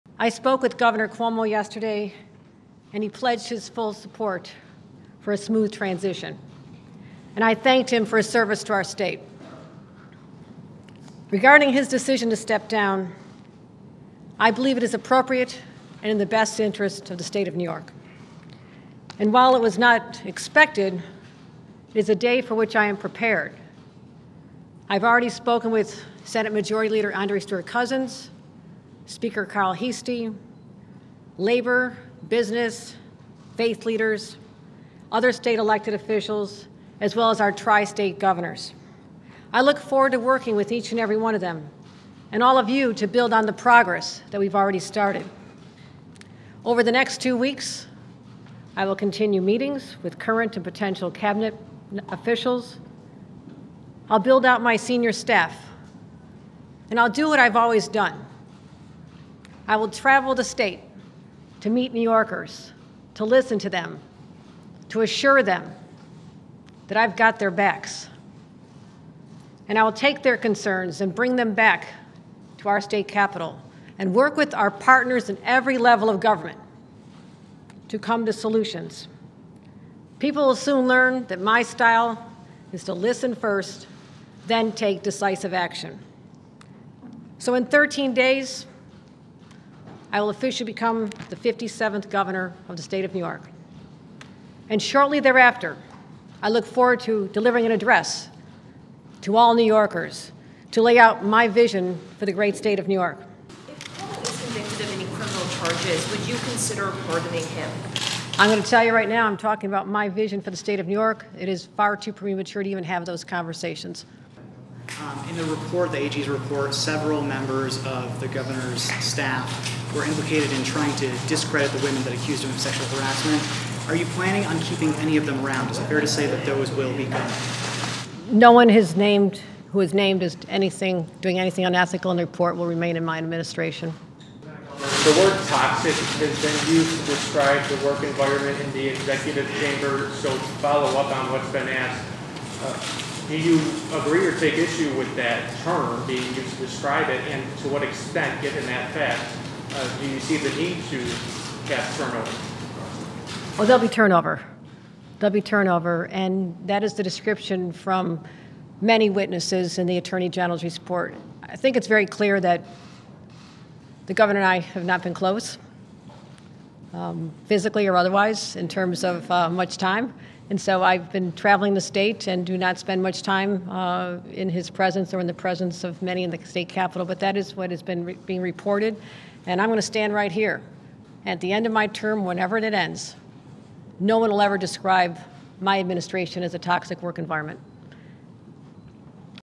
Kathy Hochul on Becoming NY Governor
'I am prepared' — Kathy Hochul, who's replacing Andrew Cuomo as NY governor, delivered these remarks about stepping into the role.